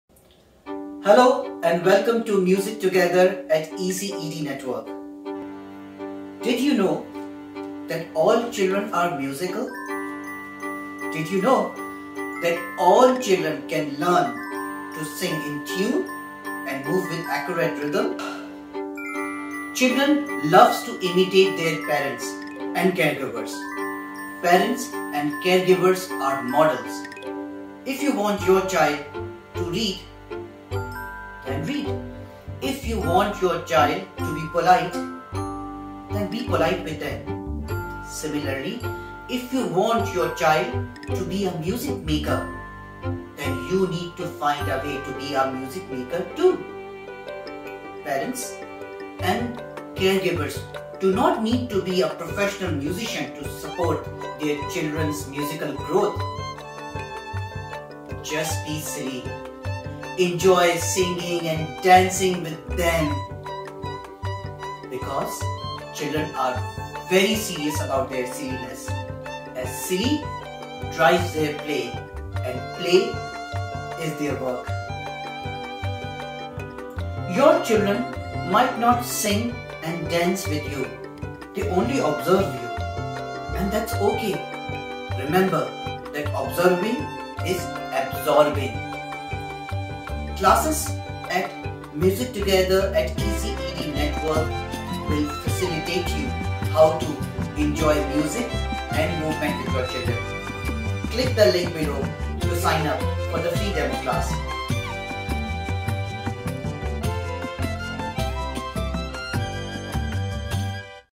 Why Music and Movement for young Children
Importance of Music and Movement for young Children
Did you know that all children are musical? ​Did you know that music strengthens your little one's brain like nothing else? Parents' musical modelling is important for children's musical growth. Join the free face to face or online demo class to enjoy singing and dancing with your child